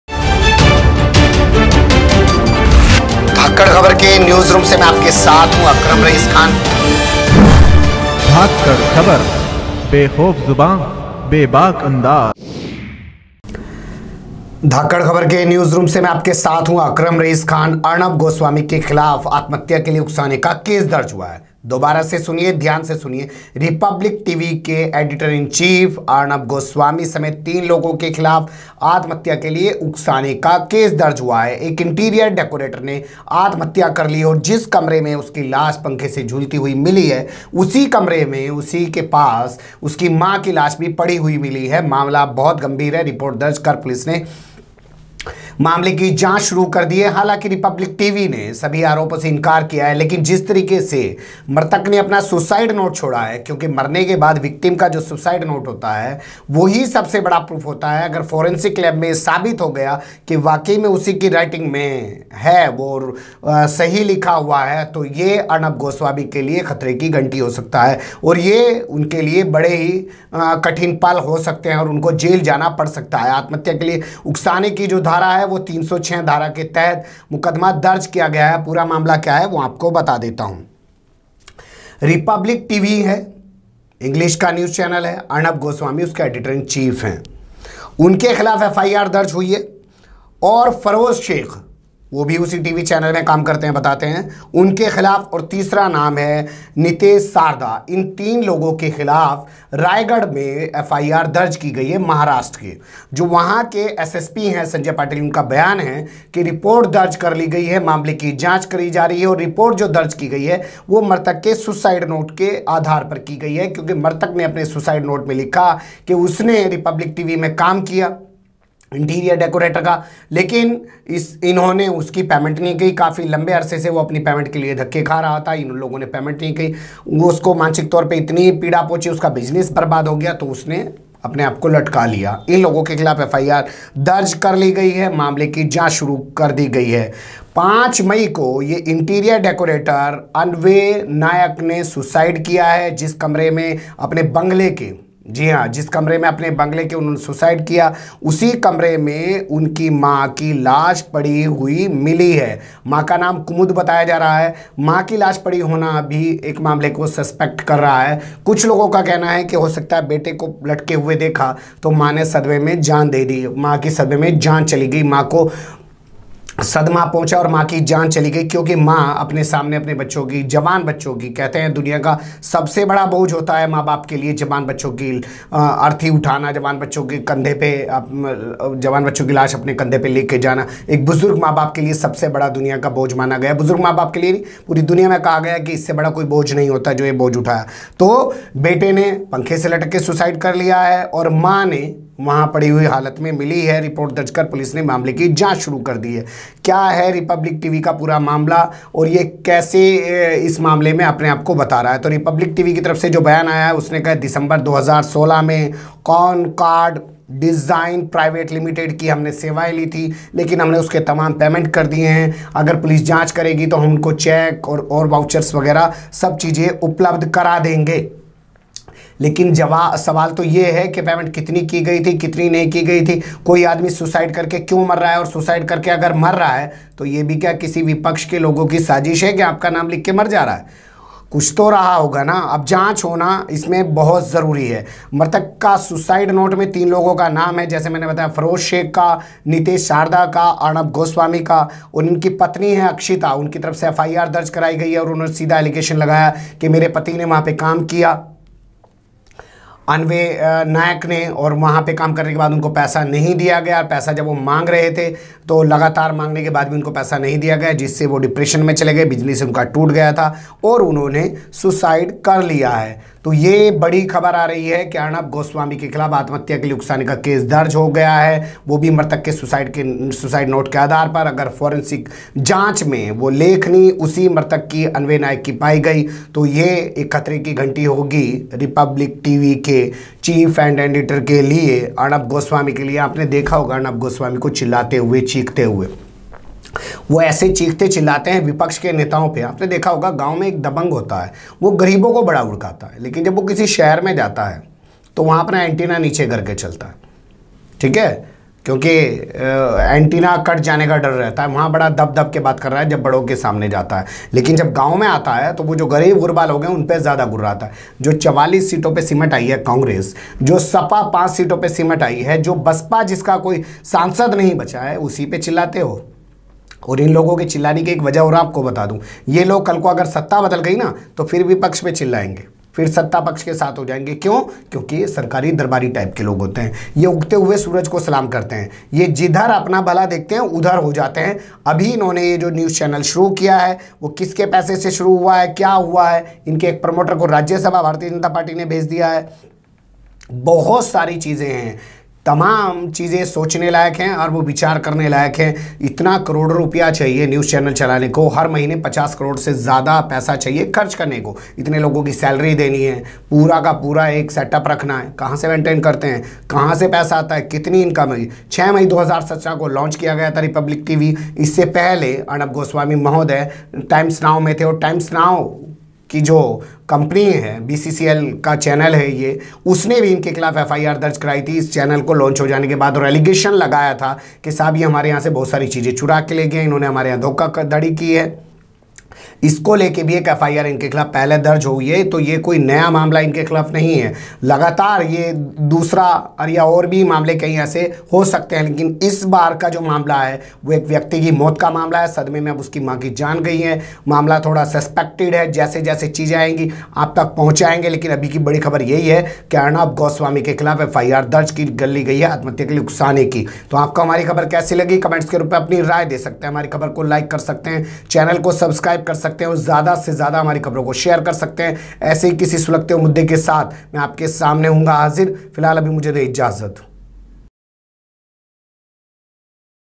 रिपब्लिक टीवी और अरनब गोस्वामी पर दर्ज हुआ हत्या का केस ! देखिए क्यों
जिस पत्रकार अरनब गोस्वामी को आप हर रोज़ शाम ढलते ही टीवी पर चीखते-छिलाते हुए देखते हैं और सिर्फ विपक्ष के नेताओं पर दहाड़ते हुए देखते हैं, उन पर एक इंटीरियर डेकोरेटर को आत्महत्या के लिए उकसाने का महाराष्ट्र के रायगढ़ में केस दर्ज हुआ है